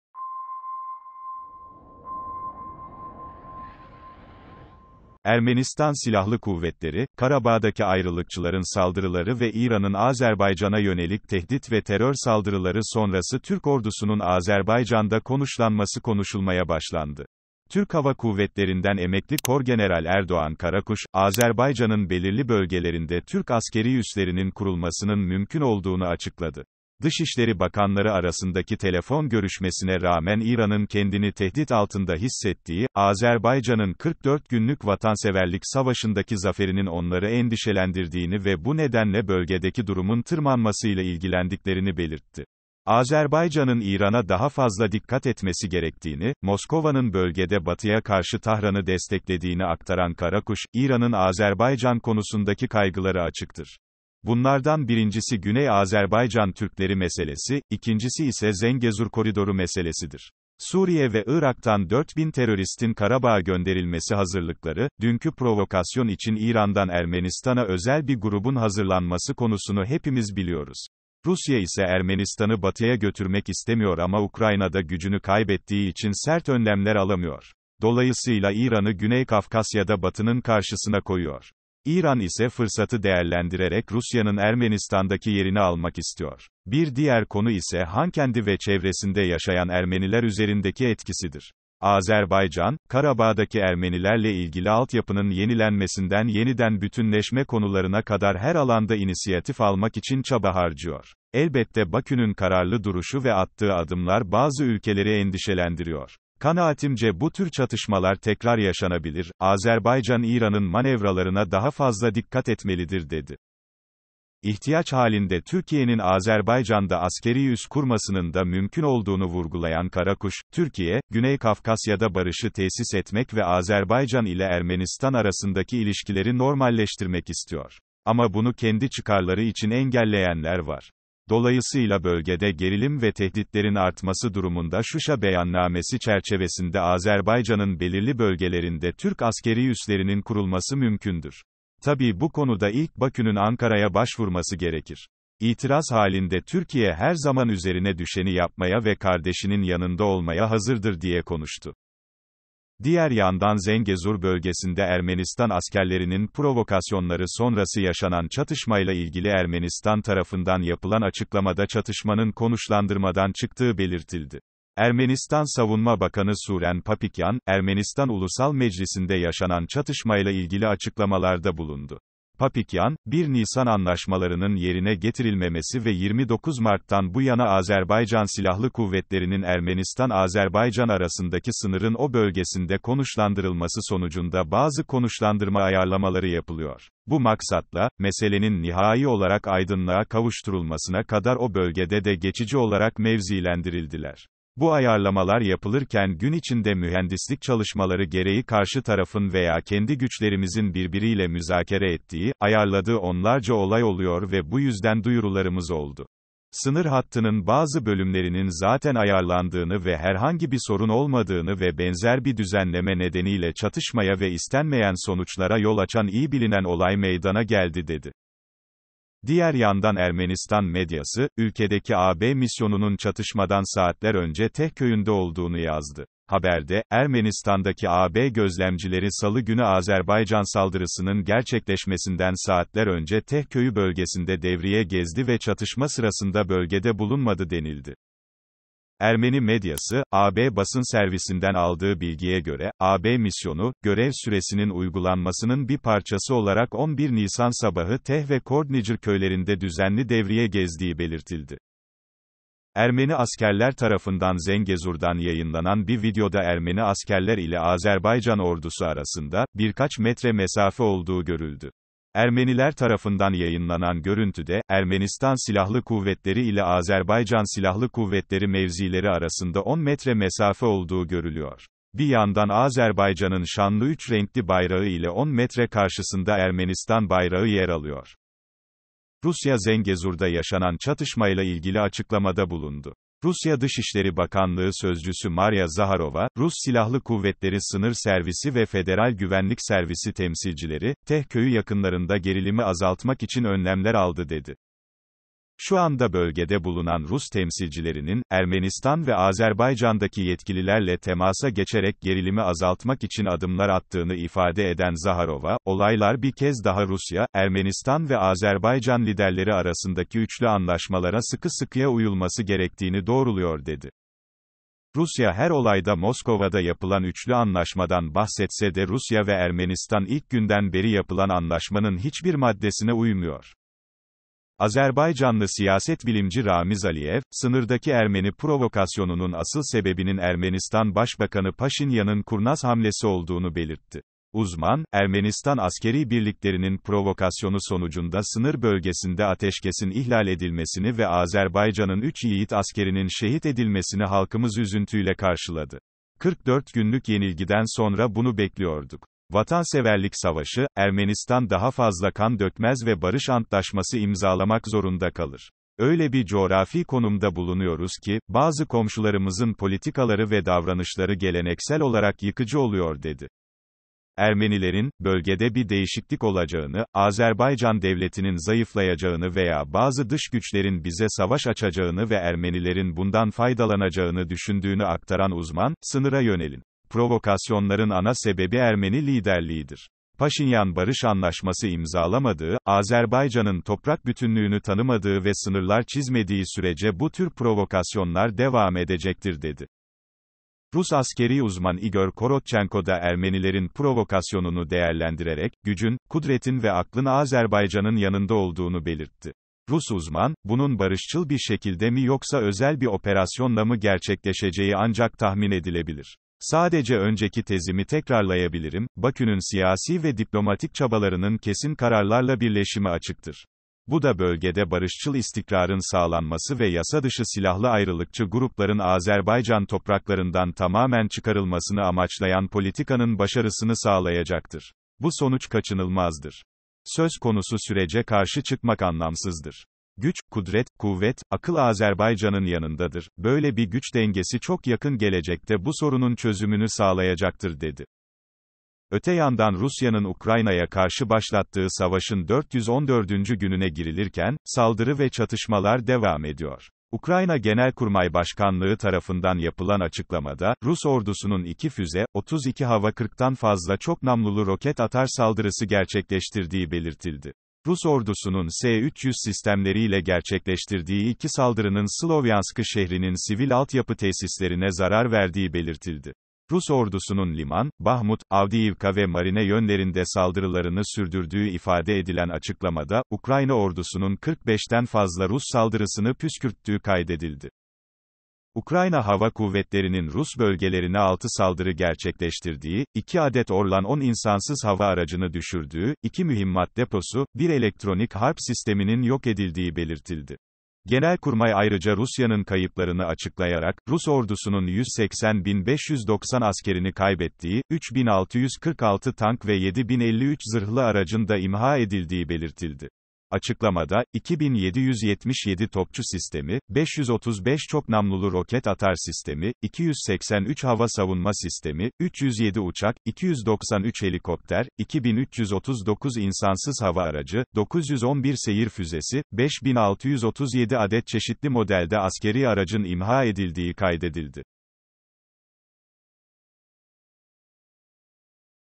TÜRK ORDUSU AZERBAYCAN’A KONUŞLANACAK | AZERBAYCAN-ERMENİ ASKERLER BUR BURUNA |TÜRKİYE AZERBAYCAN’DA
Ermenistan Silahlı Kuvvetleri, Karabağ’daki ayrılıkçıların saldırıları ve İran’ın Azerbaycan’a yönelik tehdit ve terör saldırıları sonrası Türk ordusunun Azerbaycan’da konuşlanması konuşulmaya başlandı.